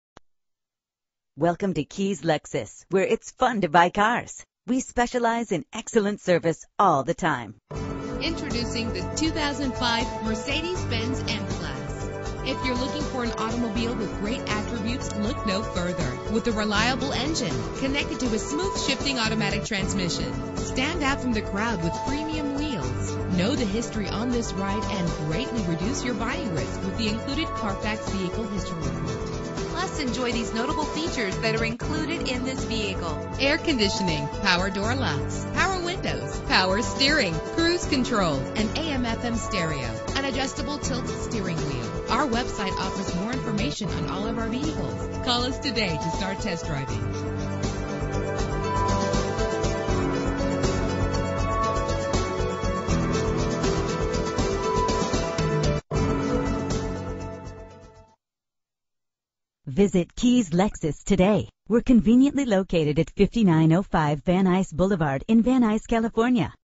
2005 Mercedes-Benz M-Class - Van Nuys CA
Year: 2005
Make: Mercedes-Benz
Model: M-Class
Trim: ML350 Sport Utility 4D
Engine: 6 cylinder 3.7 Liter
Transmission:
Color: Gold
Mileage: 87542
Address:
5905 Van Nuys Blvd